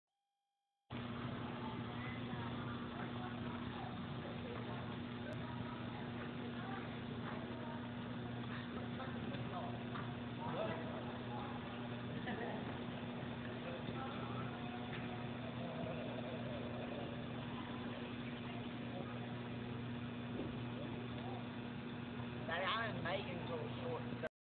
Peaceful Valley Farms Certified Natural Horsemanship Training
Battal and Dixie at the lope after their warm up.  Friday night in the arena before Parade of  Breeds